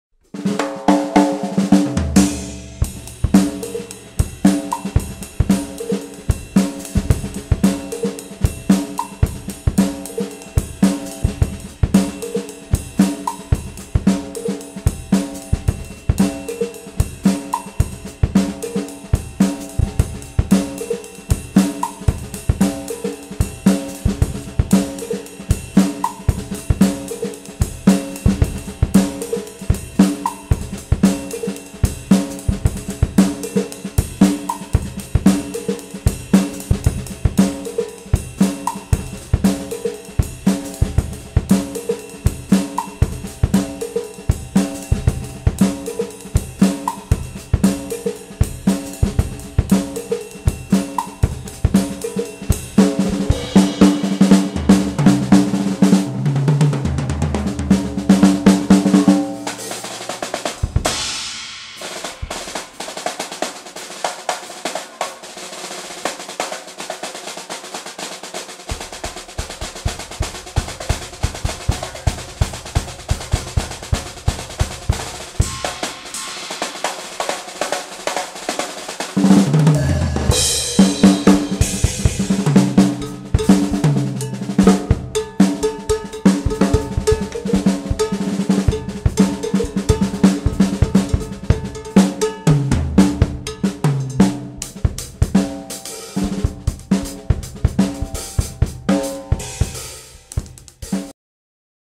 Multipedal fun
Mientras estudiaba, surgió este ritmo y me dije: ¿porque no grabarlo?. Así que ahí va...